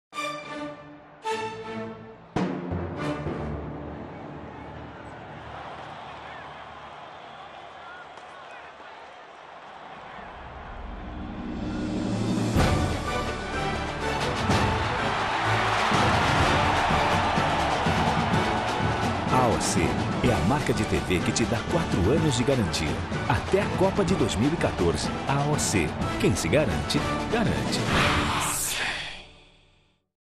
Campanha da Giacometti para a AOC inova e traz show de tecnologia
Com um show de tecnologia e uma super produção, a campanha criada pela Giacometti Propaganda, Branding e Arquitetura de Negócio® para a AOC destaca a garantia de 4 anos que a fabricante está dando para seus televisores. O filme Jogo, peça principal da campanha, foi produzido com avançada tecnologia no que se refere à iluminação e à captação de imagem. É o primeiro no Brasil a utilizar um refletor de luz com potência de 50.000 watts, que imita a luz do sol, com temperatura de cor de 5.600 graus Kelvin. Acima destas cifras, no Brasil, só existe mais potência nas radiações de luz do próprio Sol. Em cartaz até 11 de julho de 2010.